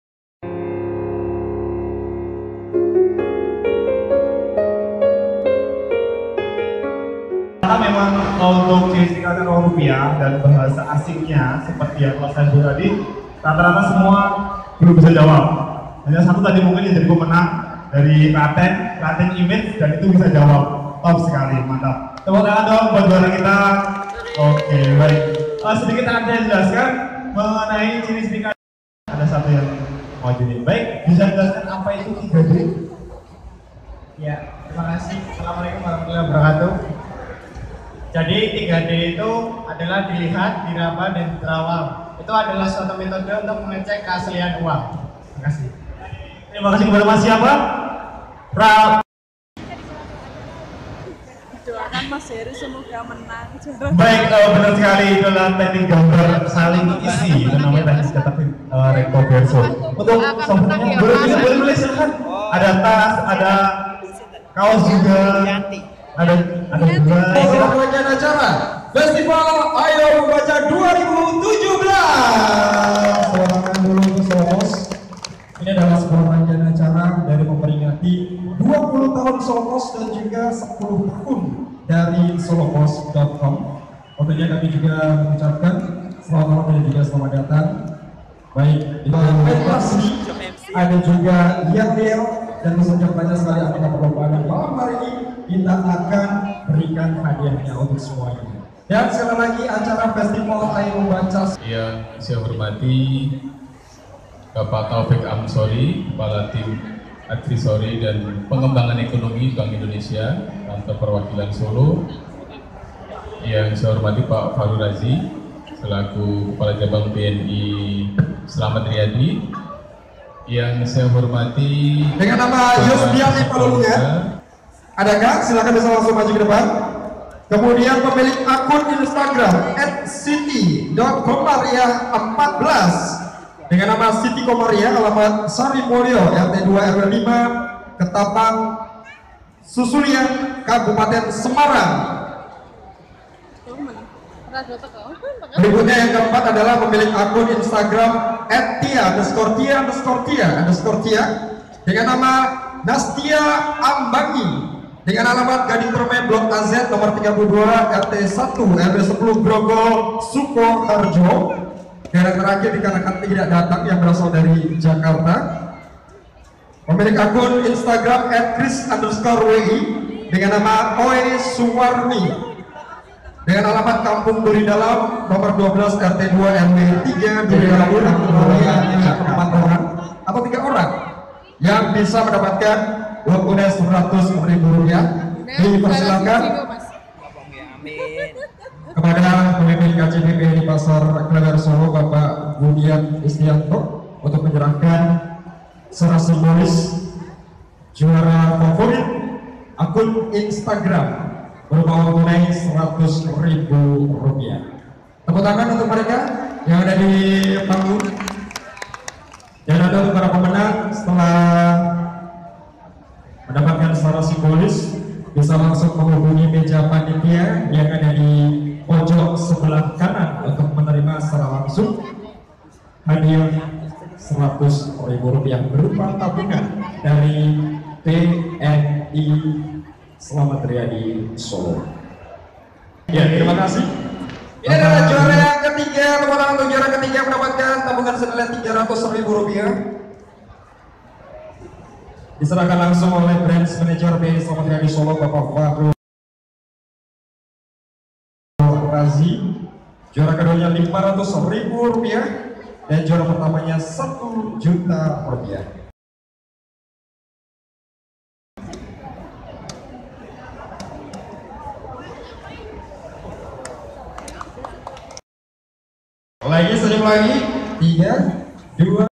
Penyerahan hadiah lomba "Foto Ceria" HUT Solopos ke 20 ( 17 September 2017 )
Menjadi bagian dari moment Ultah Solopos ke 20 adalah suatu kebanggaan. apalagi menjadi salah satu pemenang dari lomba foto ceria meski hanya sebagai pemenang harapan. tapi cukuplah sbg hsl jerih payah kami. dari Komunitas Penggemar Rajut Talikur ( KaPeErTe ) foto kami saat kunjungan pada Open House Solopos.